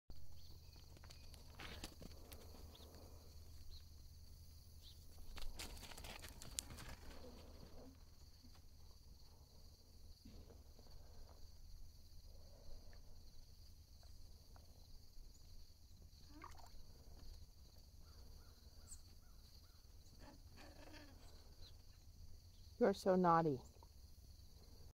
Loki - September 8th, 2022
Loki is growing like a weed. 10 weeks old, 43 lbs. And still an adorable tiny demon to the others. The light fawn lady, Bit of Honey, is his favorite to "play" with. She's was Chips mom and still has way more patience for his antics then the others do!